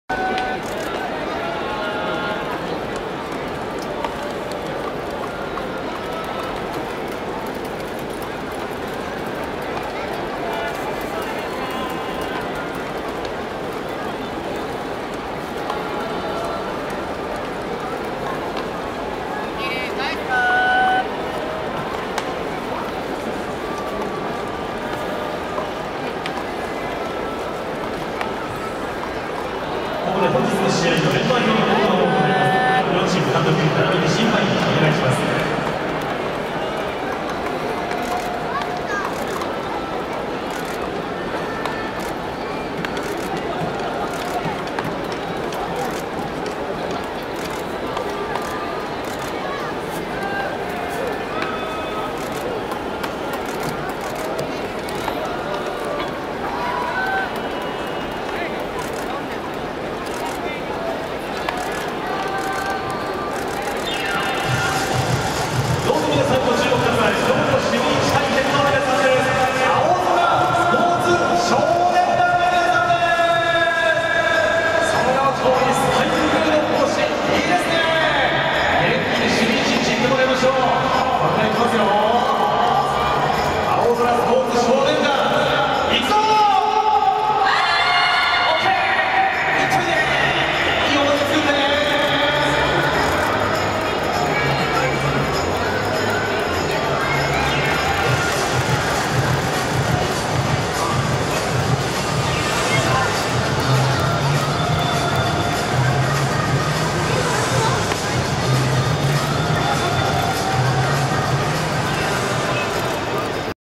16/08/21 メンバー表交換のドアラから「位置について」のシャオロンとパオロンも
メンバー表交換から位置についてまでの様子です